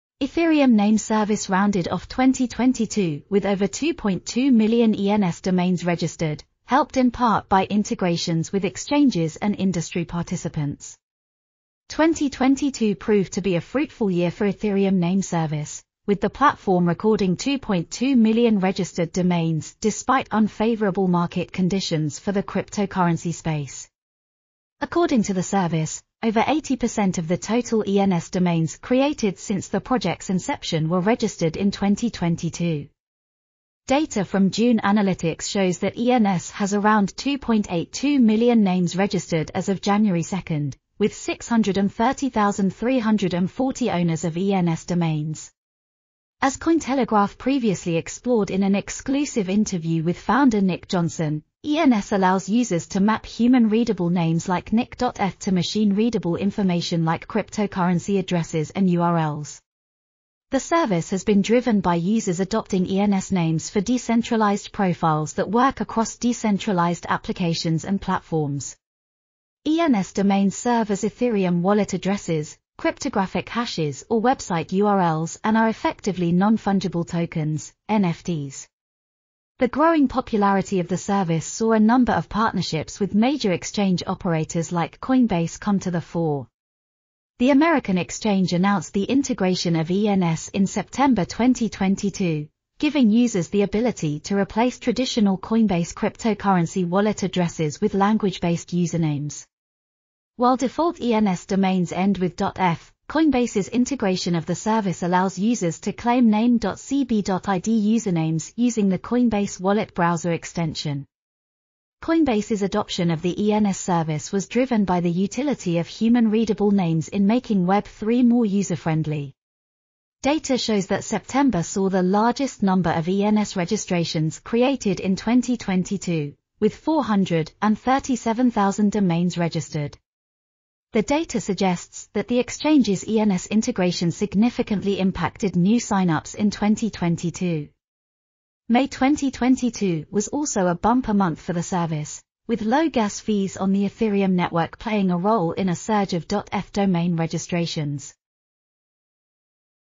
➡️ Crypto exchange adoption boosts ENS registrations to over 2.2M
Ethereum Name Service rounded off 2022 with over 2.2 million ENS domains registered, helped in part by integrations with exchanges and industry participants.